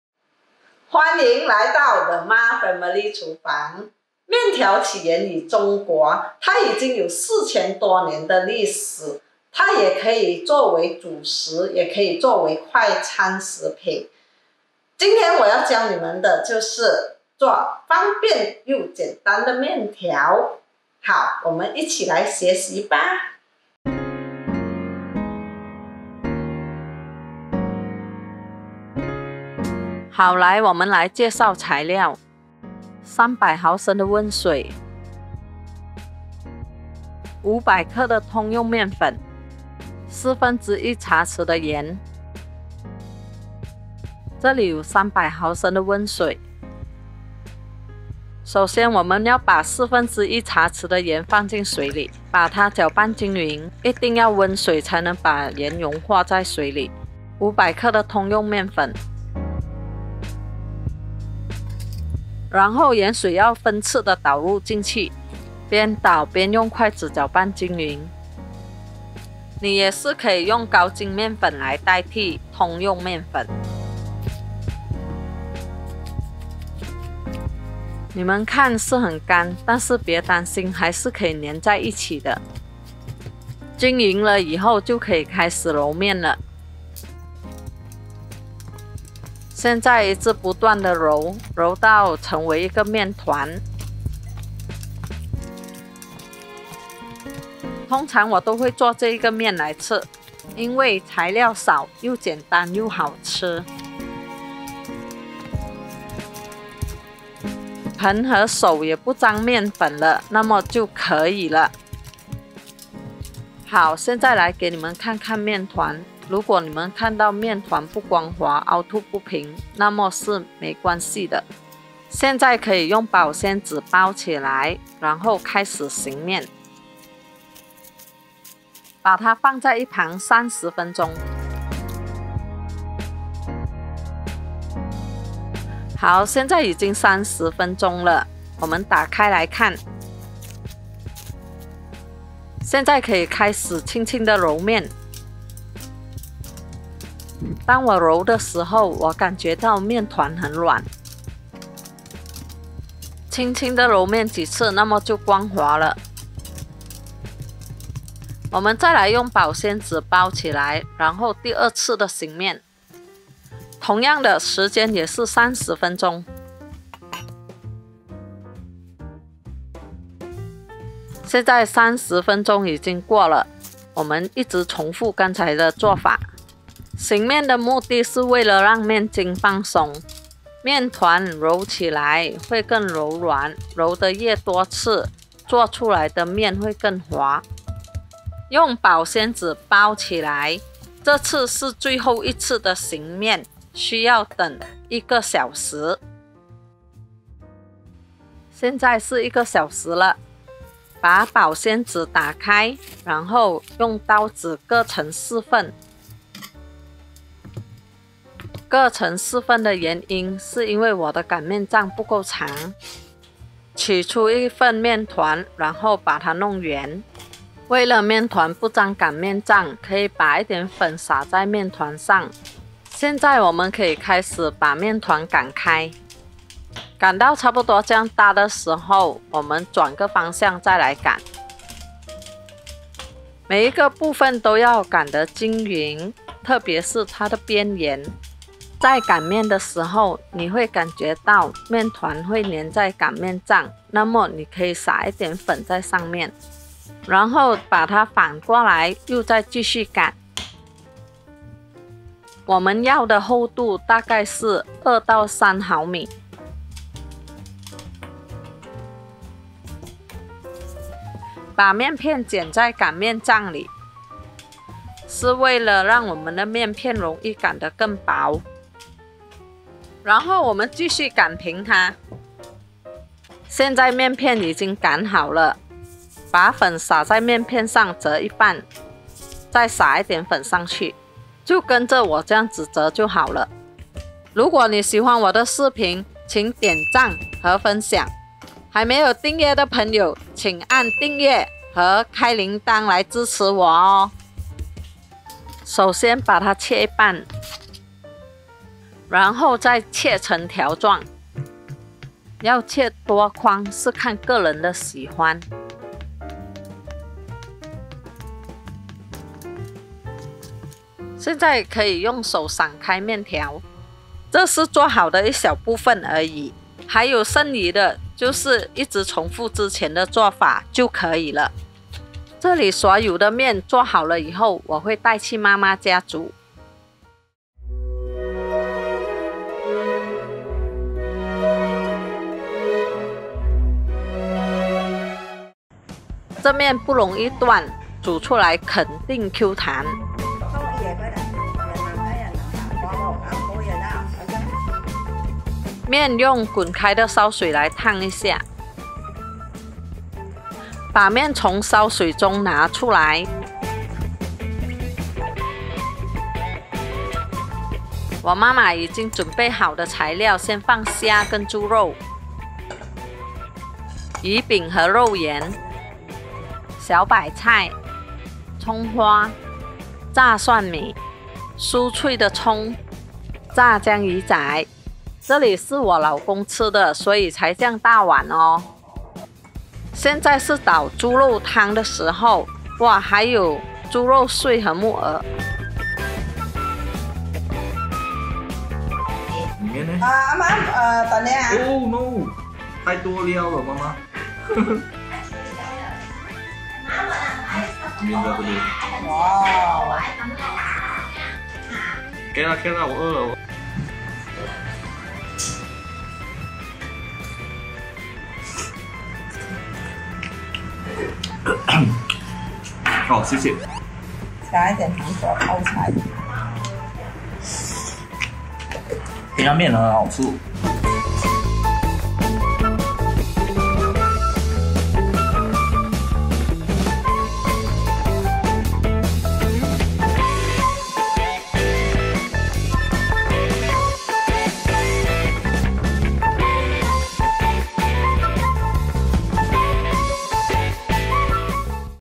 🍜 QUICK & EASY Handmade Noodles 【快速简单的刀切面】
Welcome to the Mah Family (Kitchen) Channel. Today, I'm going to be making some Homemade/Handmade Noodles! There are many ways to make noodles. This is one of the most easiest, simplest and common way to make noodles yet still so delicious. So let's get cooking!

嗨，大家好！今天我要制作刀切面！面条的做法有很多种。这是制作面条最简单和最常见的方法之一，但仍然很好吃。我们开始吧！

🍽️ Ingredients / 材料 🥢
- 300mL of warm water / 300毫升温水
- 500g of all-purpose wheat flour / 500克通用面粉
- 1/4 teaspoon of salt / 1/4茶匙盐

*You could use bread flour instead of wheat flour
*你可以用高筋面粉代替通用面粉

⏲ Timeline / 时间线 ⏲
0:00 Introduction / 介绍
0:29 Ingredients Used / 使用的成分
0:43 Making the Dough / 制作面团
1:23 Kneading the Dough / 揉面团
3:18 Rolling the Dough / 擀面团
4:55 Cutting the Dough into Noodles / 将面团切成面条
5:35 How the Noodles Can be Used / 如何使用刀切面
7:01 Ending / 结尾